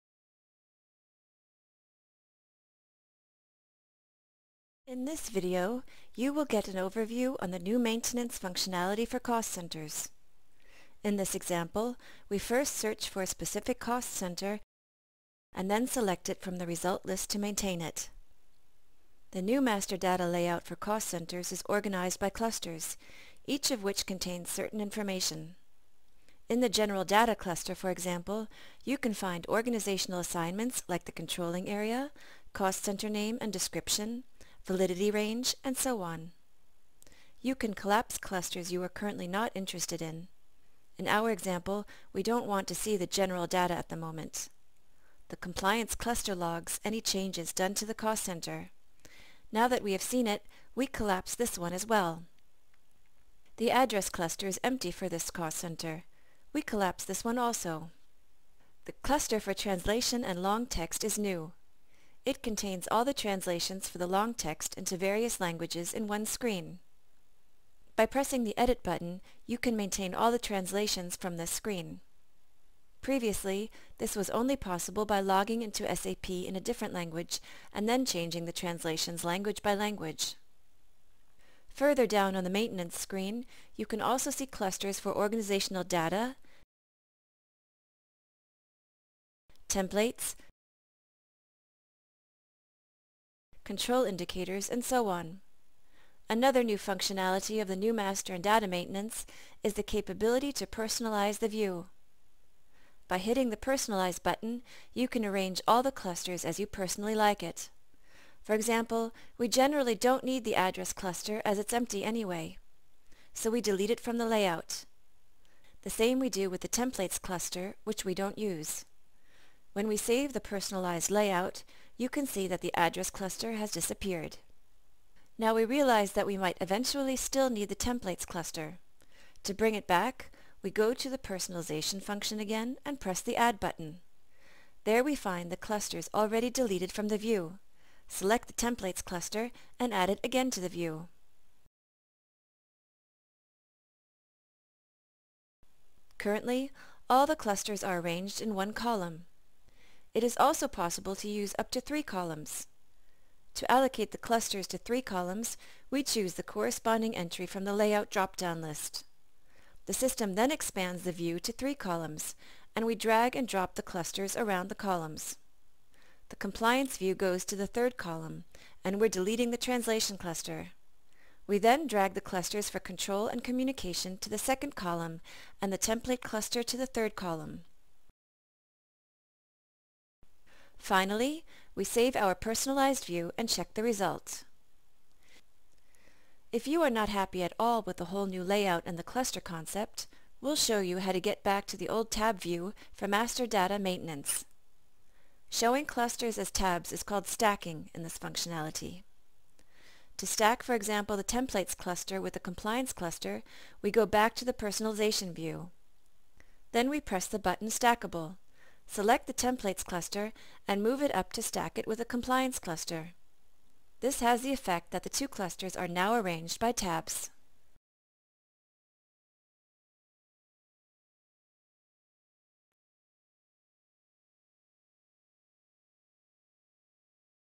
5. SAP Planning: Cost Center Master Data Maintenance
Now available:

SAP has not only improved the user interface for planning transactions, but master data maintenance is now more comfortable and flexible than before. In addition, cost center hierarchies are now time-dependent.